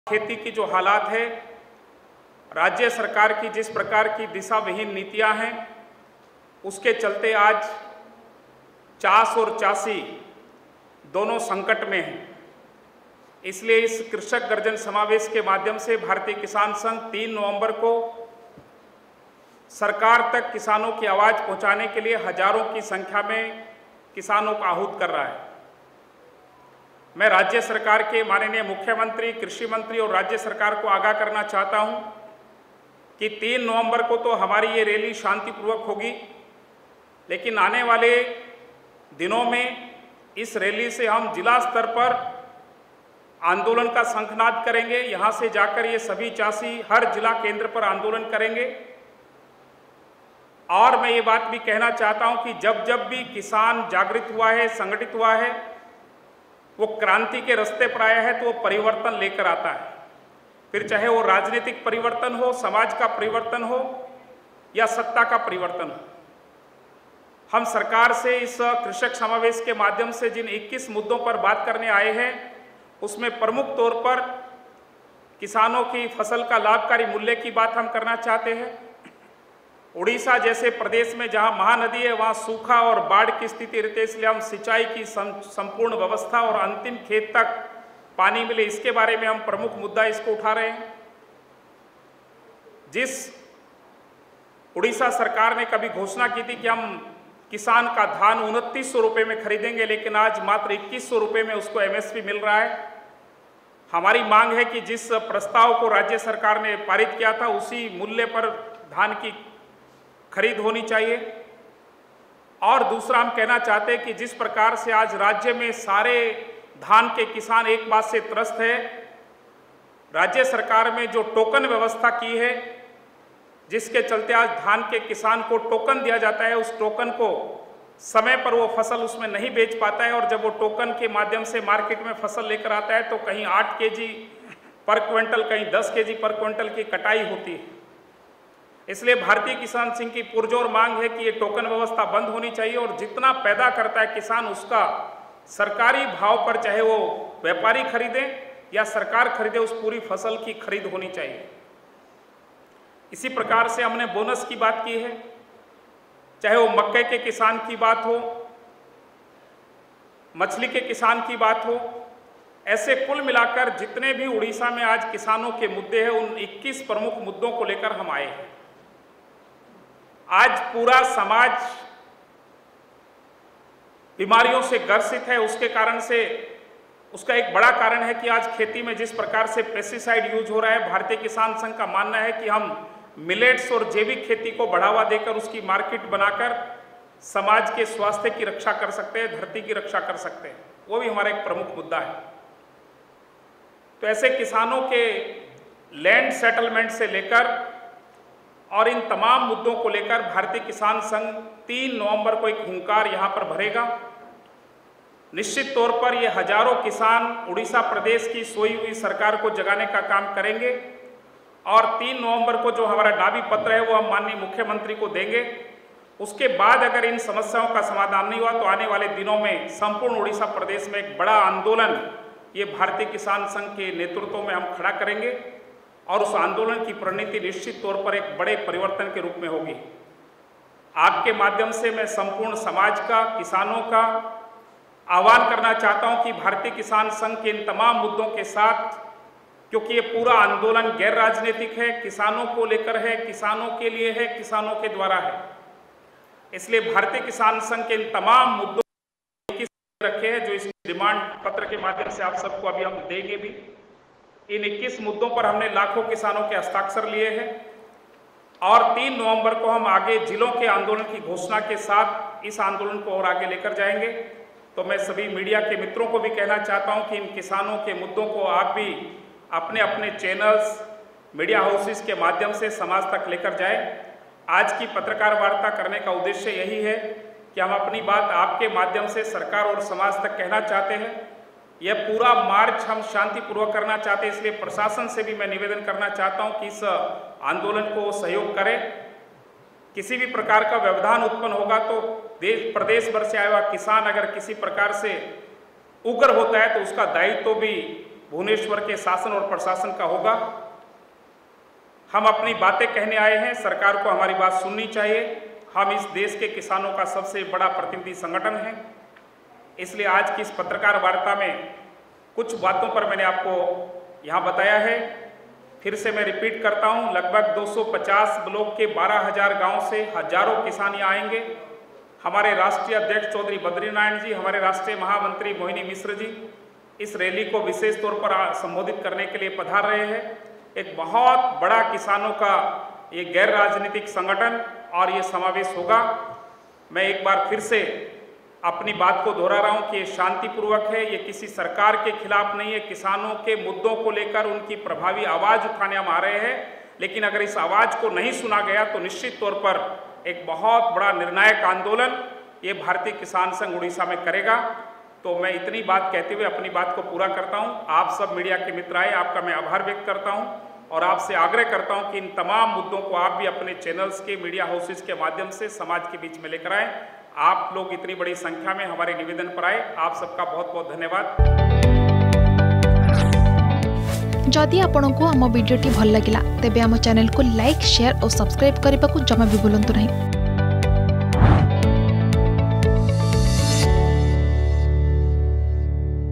ଭାରତୀୟ କିଷାନ ସଂଘର ସାମ୍ବାଦିକ ସମ୍ମିଳନୀ || Indian Farmers Union || Bhubaneswar
ଭାରତୀୟ କିଷାନ ସଂଘର ସାମ୍ବାଦିକ ସମ୍ମିଳନୀ
ନଭେମ୍ବର ୩ ତାରିଖରେ ହେବ ରାଜଧାନୀରେ କୃଷକ ଗର୍ଜନ ସମାବେଶ  ।